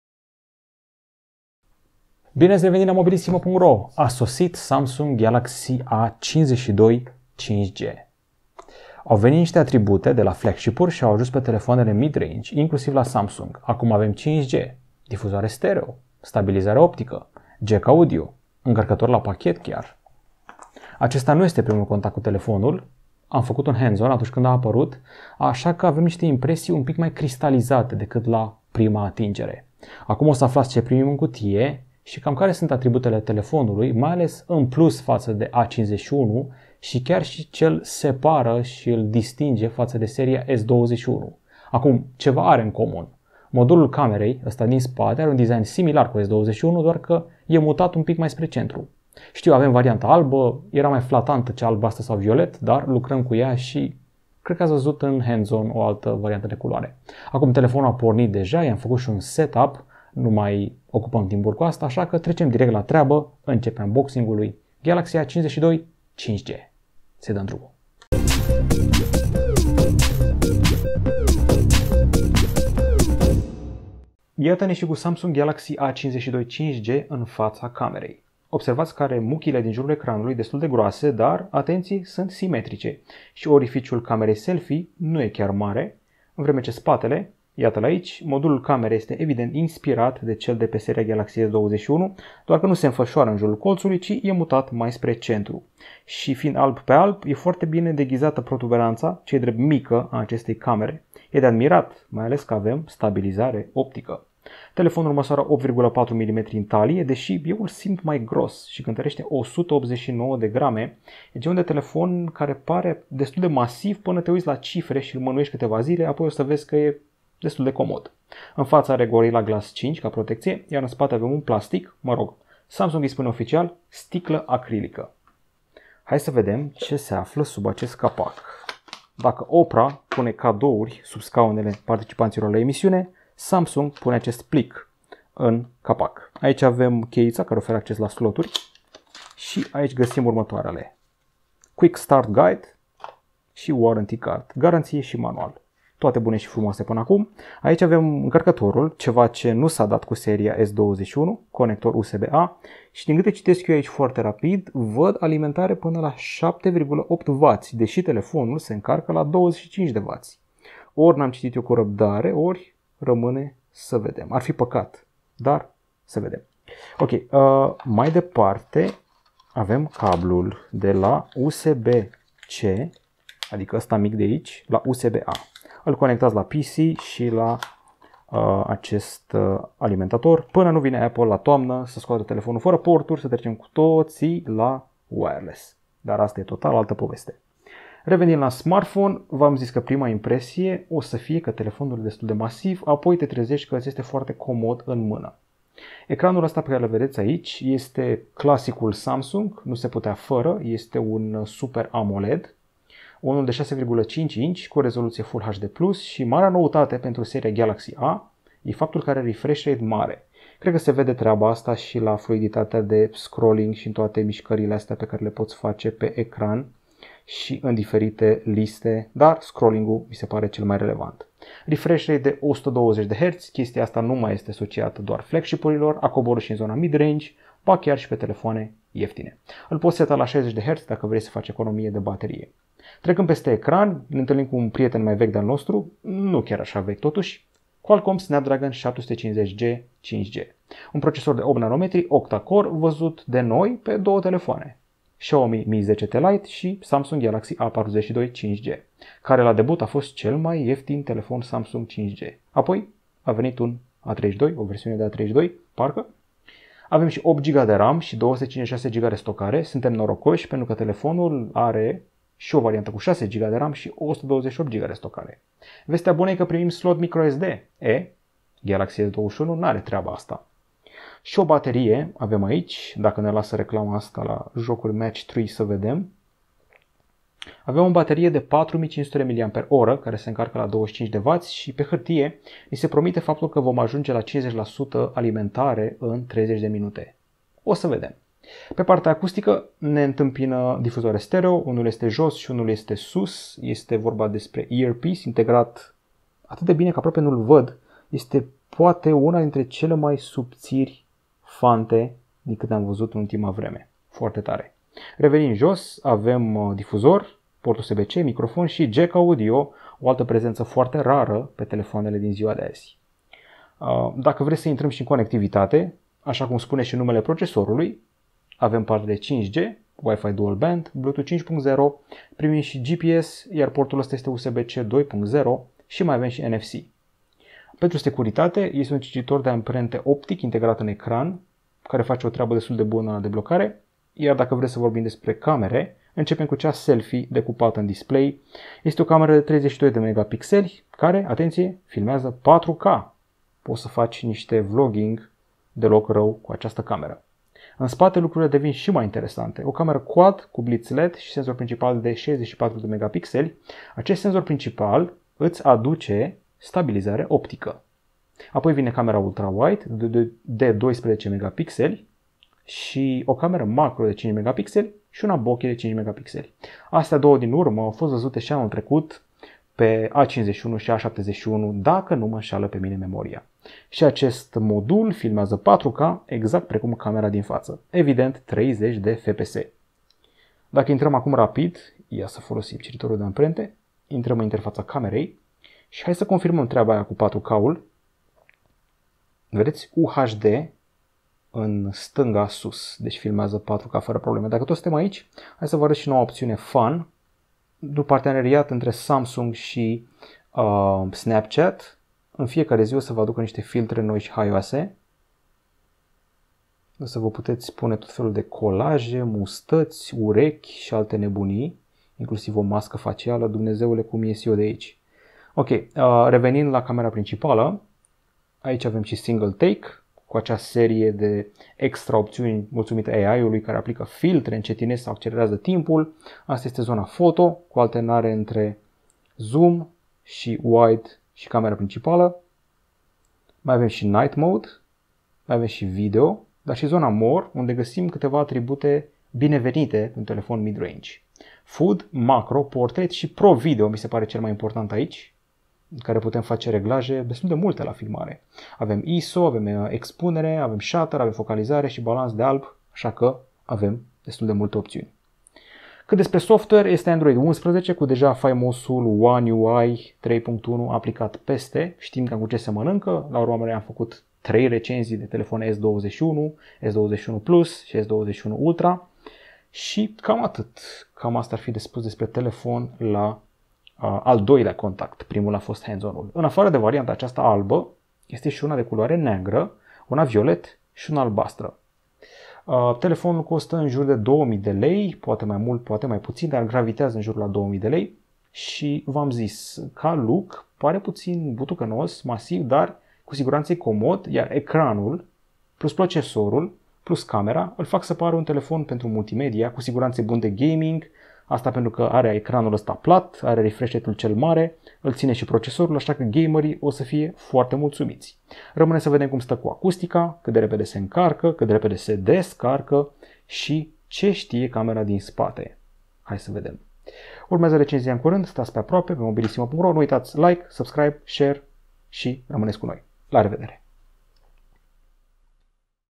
Samsung Galaxy A52 5G Unboxing în Limba Română (Ce e în cutie + extra detalii)
👁️ UNBOXING PE SITE: CURÂND

Mobilissimo.ro vă prezintă unboxing-ul telefonului Samsung Galaxy A52 5G, care costă în jur de 2000 de lei pe plan local. Aflaţi ce e în cutie, extra detalii despre dotări şi ce poate camera. Avem varianta albă aici şi facem paralele cu predecesorul A51 şi cu S21 chiar. Primim încărcător la pachet, ecran Super AMOLED de 6.5 inch cu refresh rate de 120 HZ, dar şi un procesor Snapdragon 750G puternic, alături de 6 sau 8 GB RAM. Are difuzoare stereo, baterie de 4500 mAh cu încărcare la 25W şi o cameră quad cu senzor principal de 64 MP cu stabilizare optică. Rulează Android 11 cu One UI 3.1 aplicat peste.

Samsung Galaxy A52 5G Unboxing în Limba Română